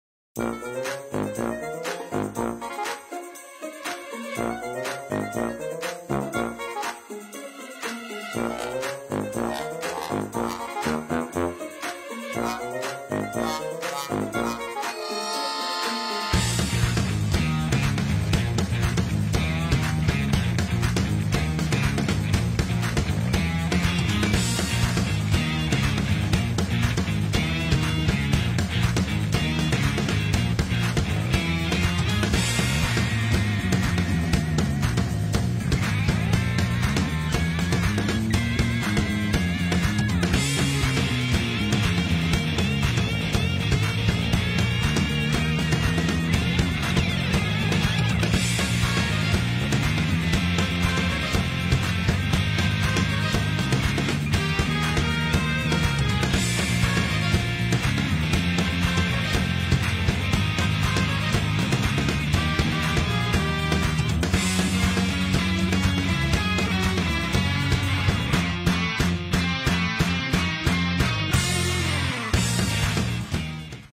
【小編開箱】水晶三眼仔ALIEN REMIX Swarovski®水晶+經典角色
【小編開箱】PIXAR變裝三眼仔的ALIEN REMIX，三眼仔上面有Swarovski®的水晶，更雲集不同《TOY STORY》電影經典角色，包括礦工彼得，索克天王，抱抱龍，桃莉，吱吱企鵝，勞蘇，綠兵和巴斯光年等！而今次的超隱藏版更是剋星阿薛！好靚啊~

【INFO】
HEROCROSS 世界英雄聯盟
早鳥價: HK$999

全方位緊貼《U Magazine》社交平台